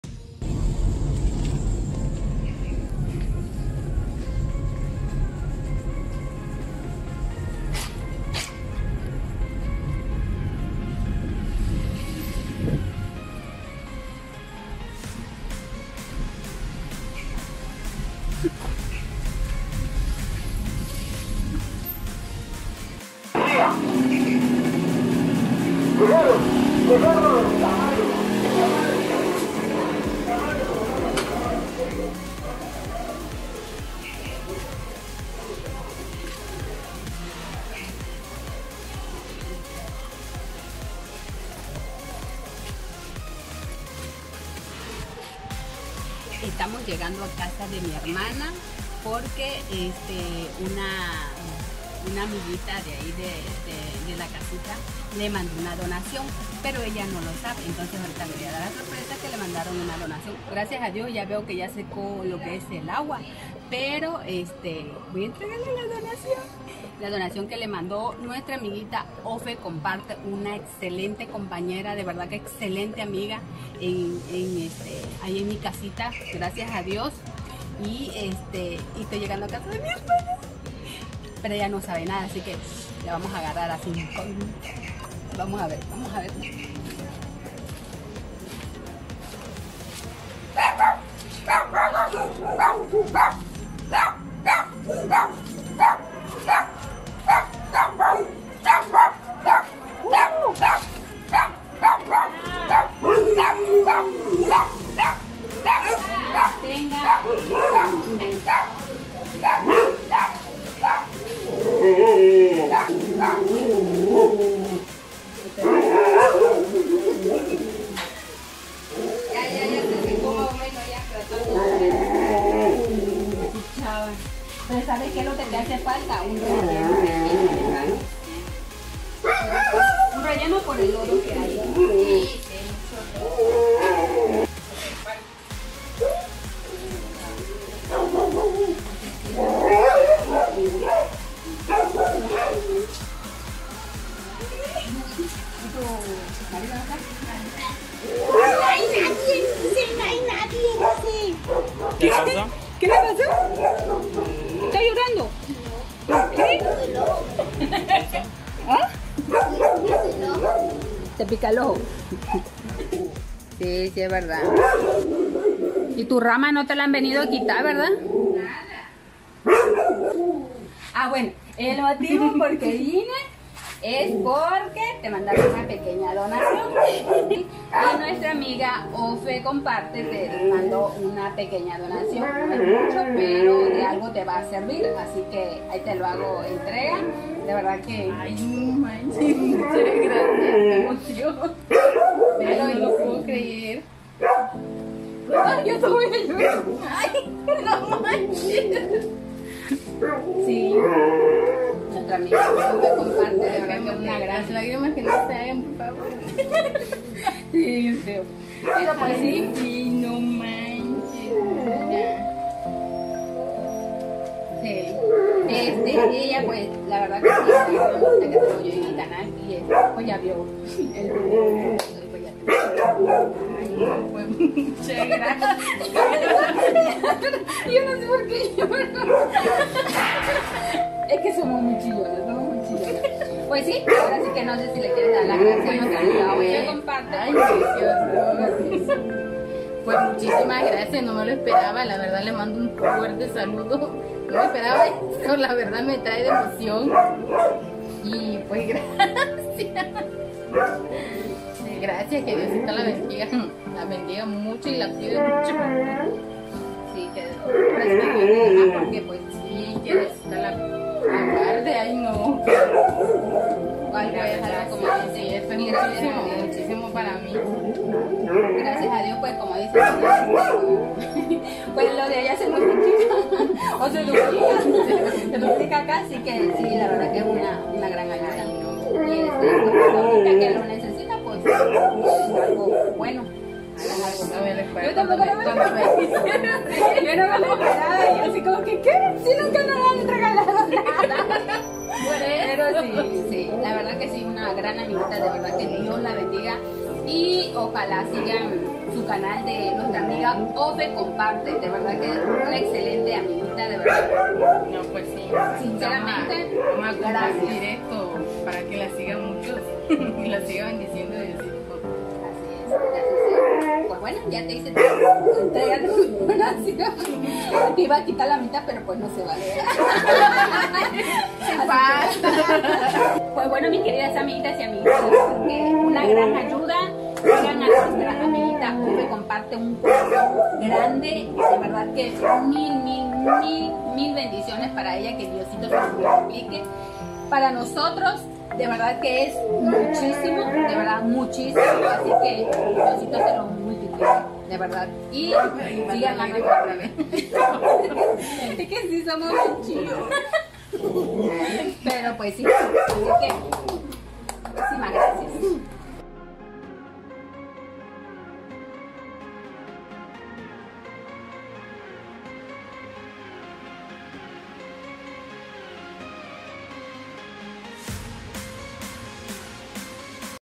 Llora al entregarle su donación.
Hola hola mis queridas amiguitas y amiguitos. Les dejo este video ya que nuestra amiguita ofe Comparte le mando una donación a mi hermana y se la fui a entregar y ella le dio sentimiento que no se la creía que alguien le mandara algo, muchas gracias bendiciones para todos 🙏😘